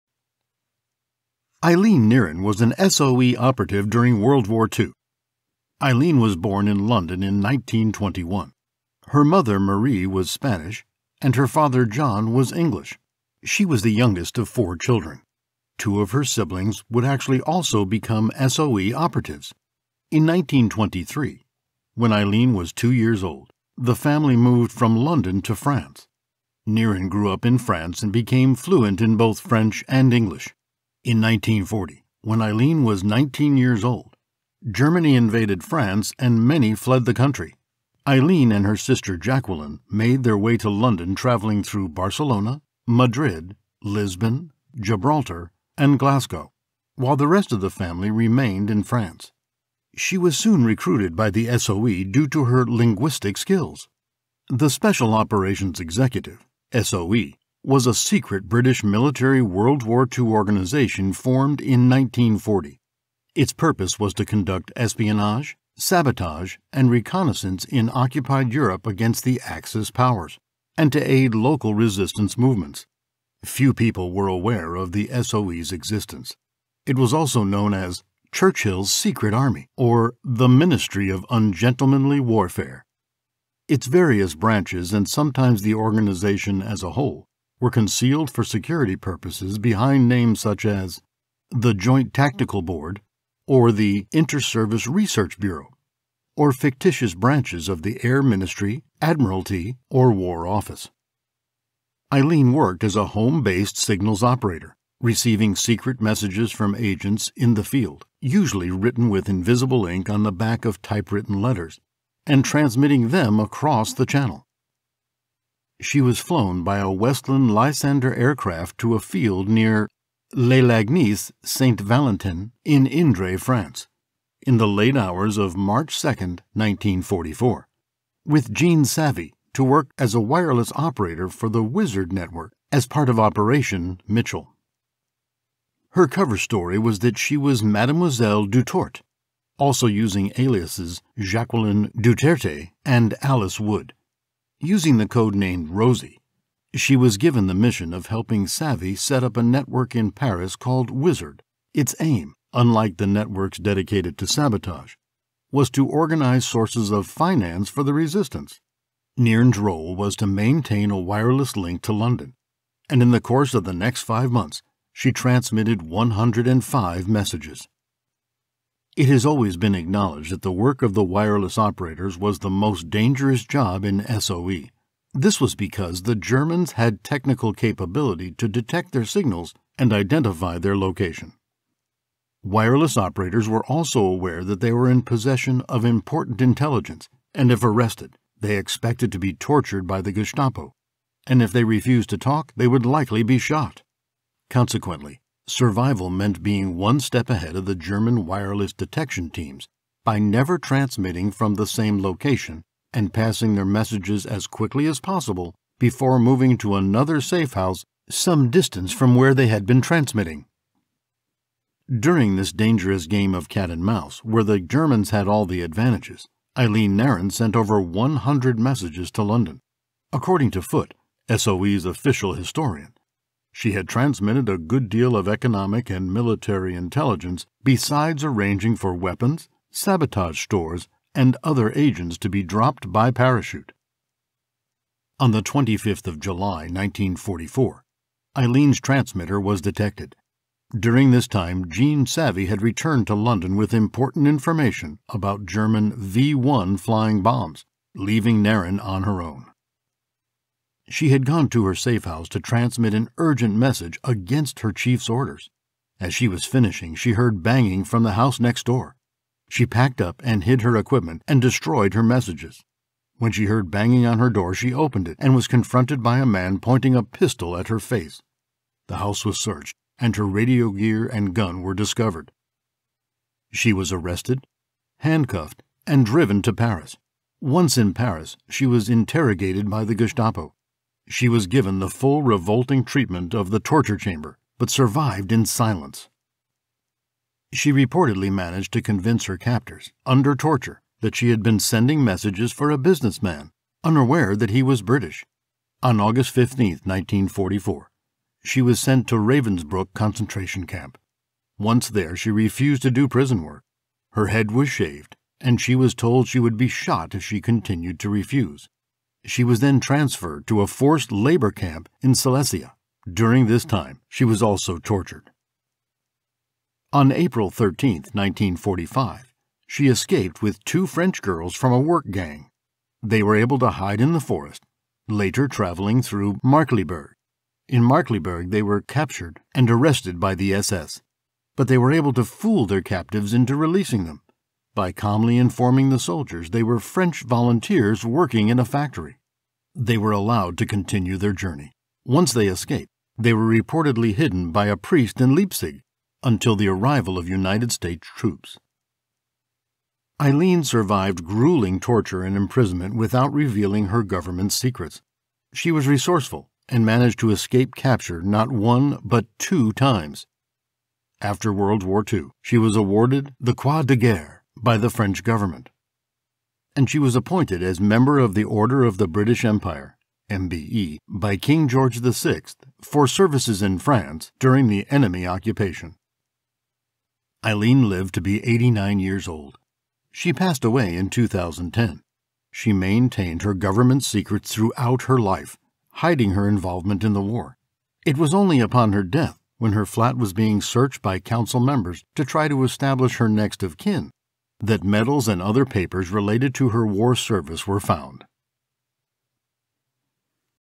The WW2 Spy that was captured and escaped twice- Eileen Nearne
Eileen Nearne was a British intelligence officer in WW2. A member of the SOE she transmitted critical information from France to Great Britain during the German occupation. Her dangerous work resulted in her being captured by the Germans. This is the inspiring true story of her loyalty and devotion to her country and her craft.